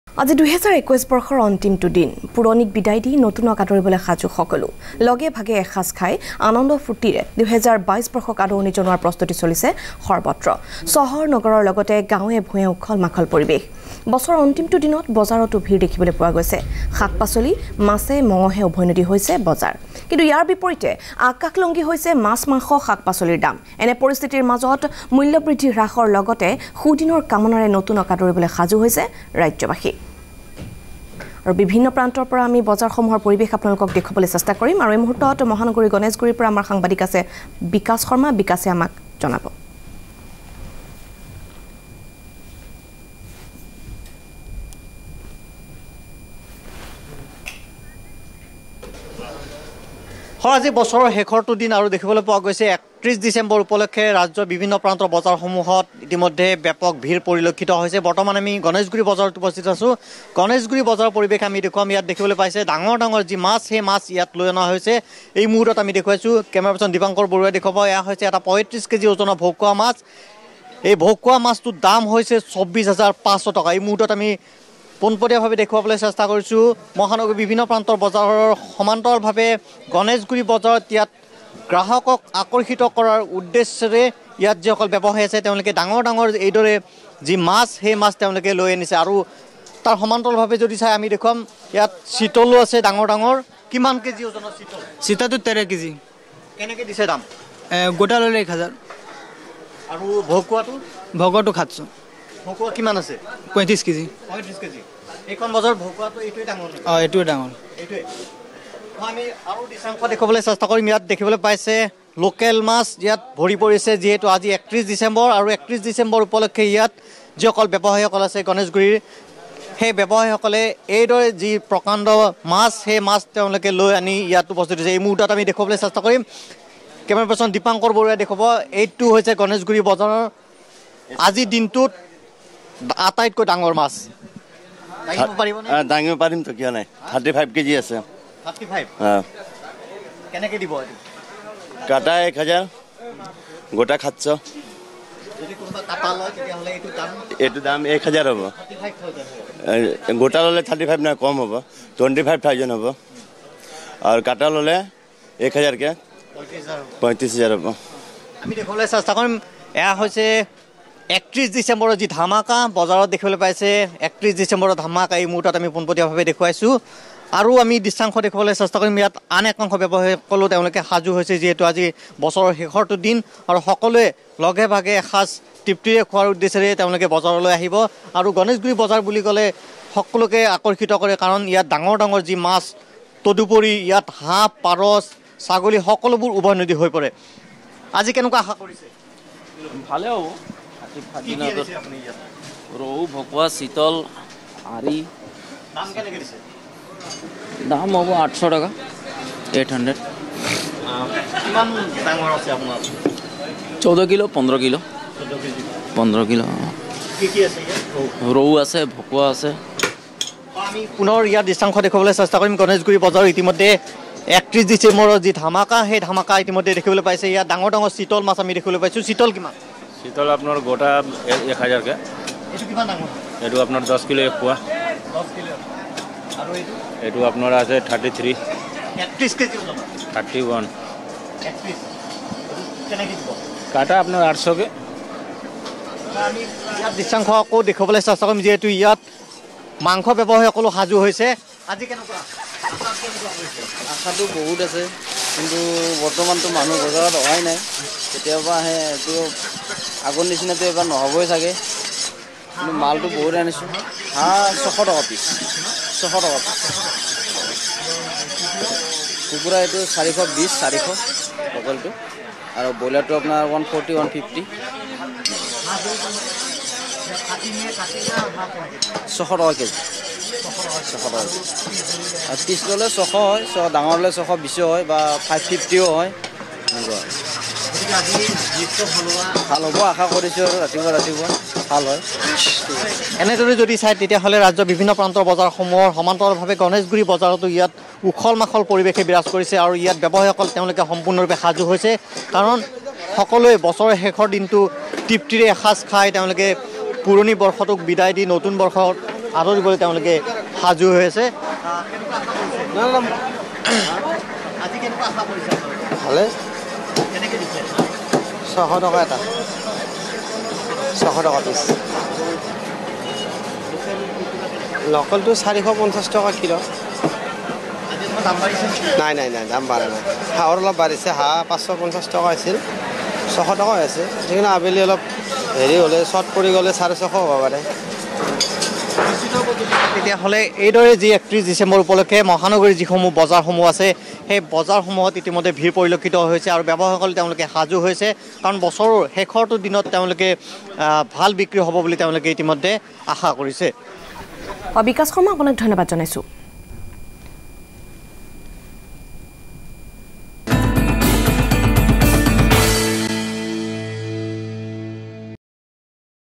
⭕️ আজি বছৰৰ অন্তিম দিন। লগে-ভাগে এঁসাজ খাই নতুন বছৰক আদৰাৰ প্ৰস্তুতি ৰাজ্যবাসীৰ।
⭕️ আজি বছৰৰ অন্তিম দিন। লগে-ভাগে এঁসাজ খাই নতুন বছৰক আদৰাৰ প্ৰস্তুতি ৰাজ্যবাসীৰ। গুৱাহাটীৰ বজাৰৰ পৰিৱেশ